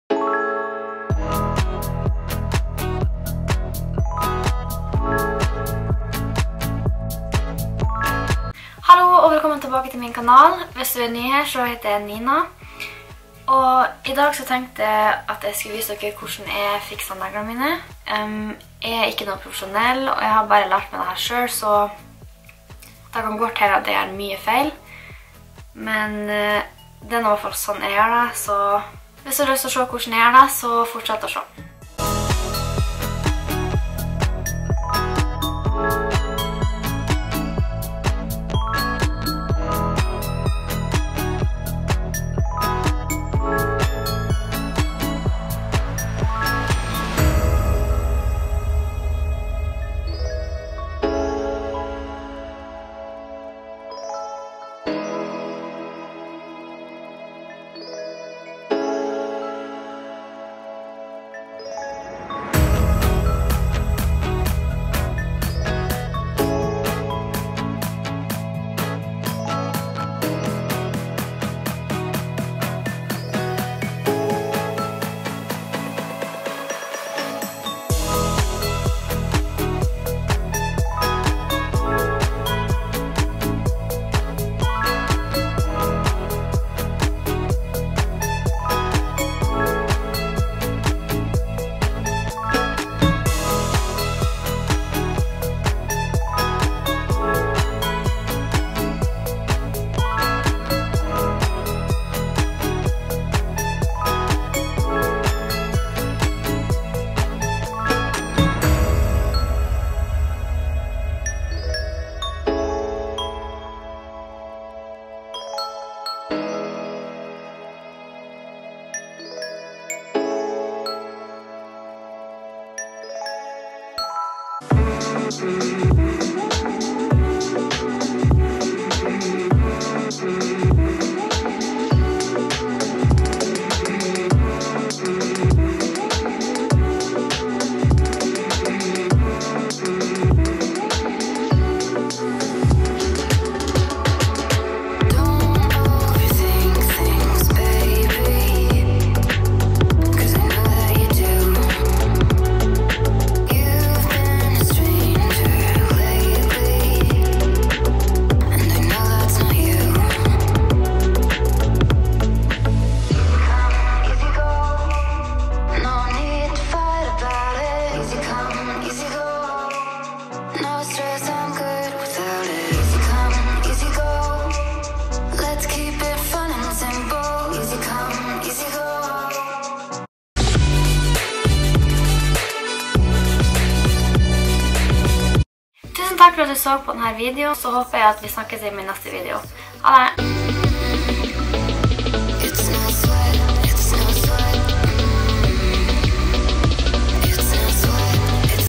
HVORDAN JEG FIKSER MINE EGNE NEGLER!
Hvordan jeg fikser neglene mine hjemme. Det tar ca 2 timer!